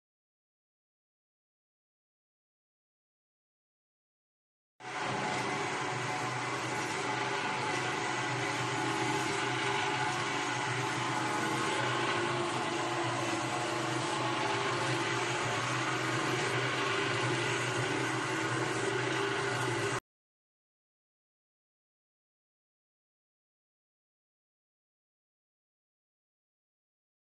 Миксер промышленный с подогревом PJ
Предлагаем вашему вниманию видео работы миксера промышленного с подогревом PJ. С полным описанием и характеристиками вы можете ознакомиться, посетив наш сайт